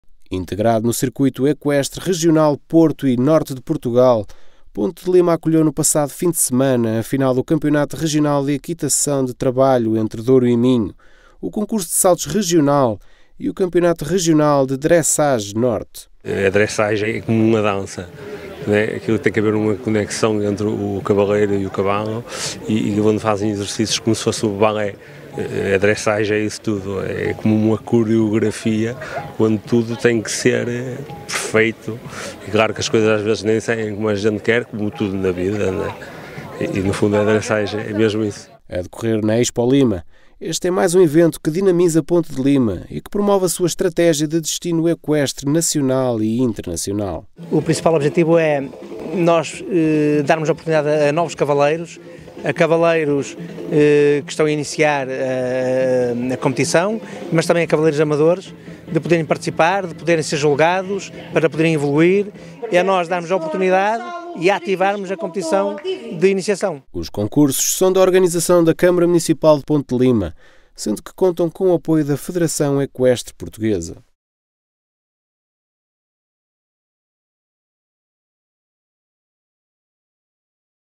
Ponte de Lima foi palco para Circuito Equestre Regional Porto e Norte | Altominho TV
Integrado no Circuito Equestre Regional Porto e Norte de Portugal, Ponte de Lima acolheu, no passado fim de semana, a final do Campeonato Regional de Equitação de Trabalho Entre Douro e Minho, o Concurso de Saltos Regional e o Campeonato Regional de Dressage Norte.

A decorrer na Expolima, este é mais um evento que dinamiza Ponte de Lima e que promove a sua estratégia de Destino Equestre nacional e internacional.

Os concursos são da organização da Câmara Municipal de Ponte de Lima, sendo que contam com o apoio da Federação Equestre Portuguesa.